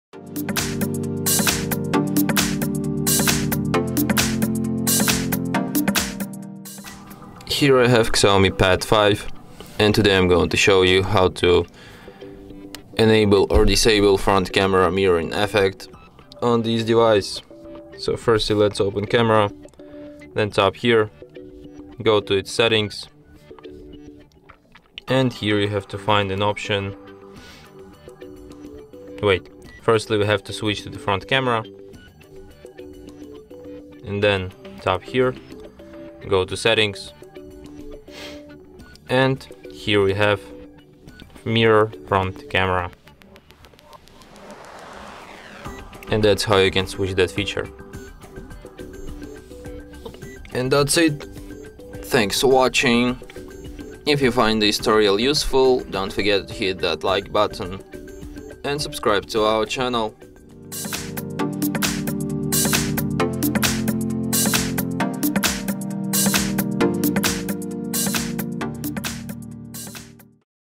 How to Activate Camera Mirroring on XIAOMI Pad 5 - Disable Camera Mirroring
Find out more info about XIAOMI Pad 5:
If you don’t like the mirror effect in your photos, you should absolutely find out how to open the camera settings and how to deactivate the mirror effect in front camera. We will show you how to turn off the mirror effect on XIAOMI Pad 5. Head over to our HardReset.info YouTube channel and check out more tutorial for your XIAOMI device.